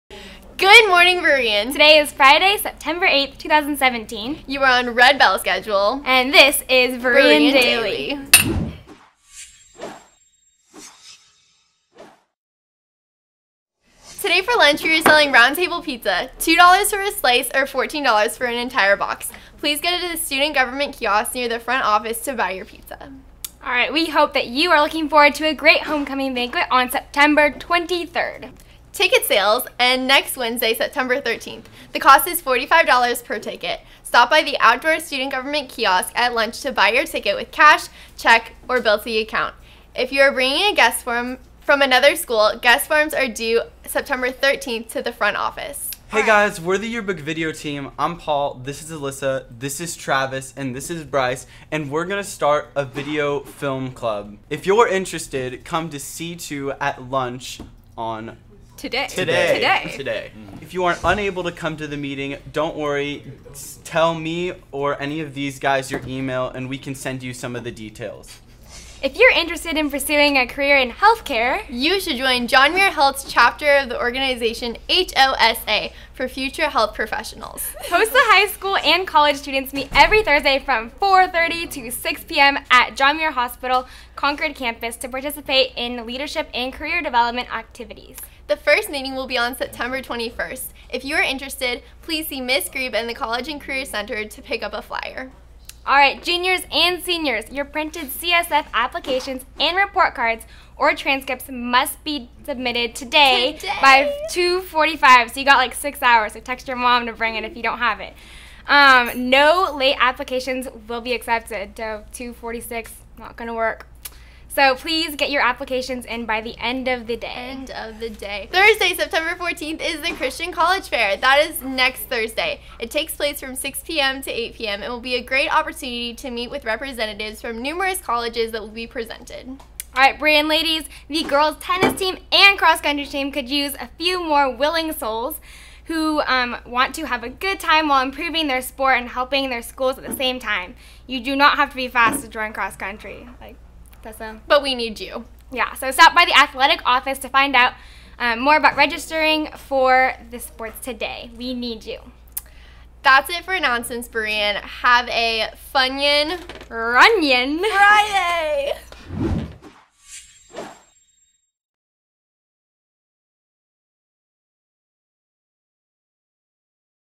Berean Daily // 09.08.17
Welcome to Berean Daily! This is a morning announcement show for Berean Christian High School.

Hosts: Tessa Bolter and Katie Leong
Camera Operator: Taylor Brost
Teleprompter operator: Bryce Alley
Editor: Bryce Joseph-Nelson

S:02 E:016